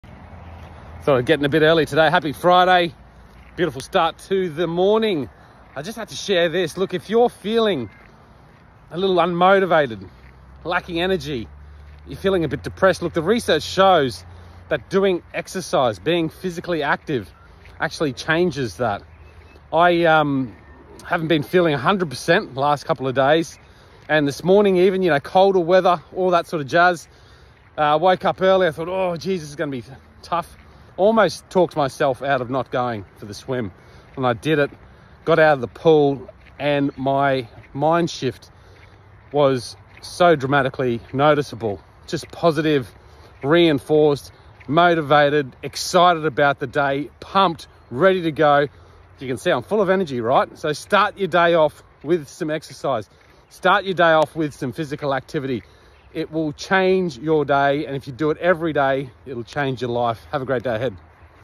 Motivational Daily-Start your day off right!! Physical activity💥Change your day, change your life!
Today's quick motivational daily talk is about the power of starting your day with physical activity. Exercise shifts your mindset, changes your energy flow, and boosts your motivation. You'll feel more excited about the day ahead. Do it today. Do it every day. Change your day, change your life! 💪